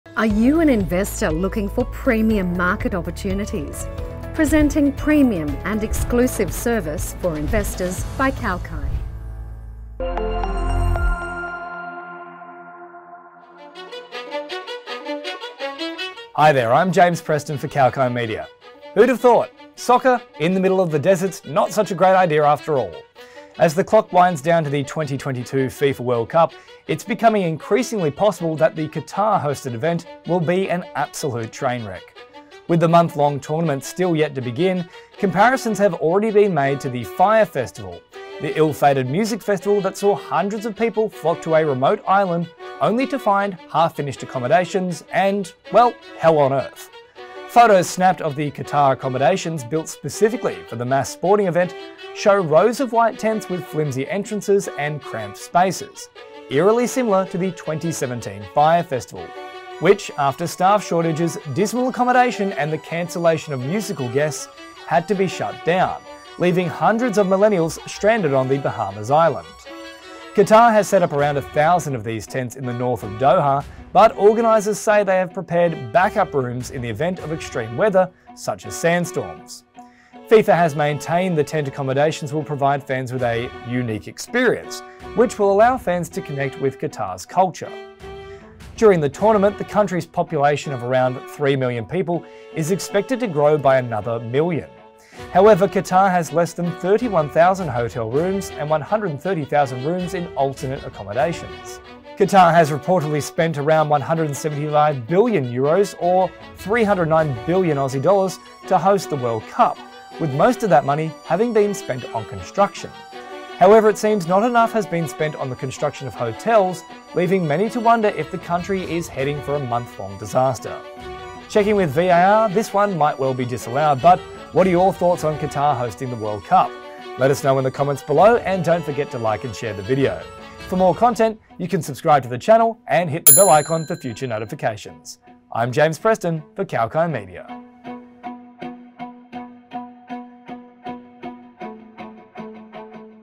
Is 2022 FIFA World Cup heading for a Qatarstrophe? | Kalkine Media
Who’d have thought? Soccer in the middle of the desert’s not such a great idea. As the clock winds down to the 2022 FIFA World Cup, it’s becoming increasingly possible that the Qatar-hosted event will result in a train wreck.